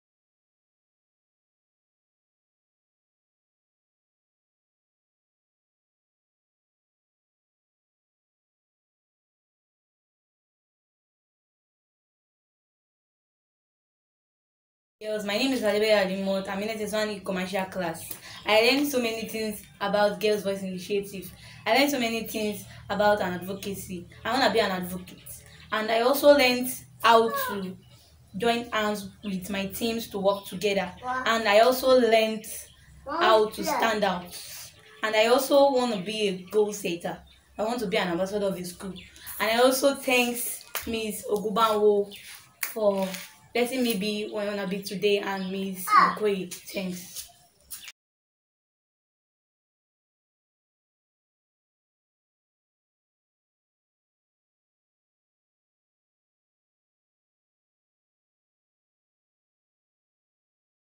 GVI Lagos Profile: ADEBAYO Alimot
A profile of Alimot Adebayo, a Girl Leader in Rise Up's Girls Voices Initiative implemented by Action Health Incorporated in Lagos.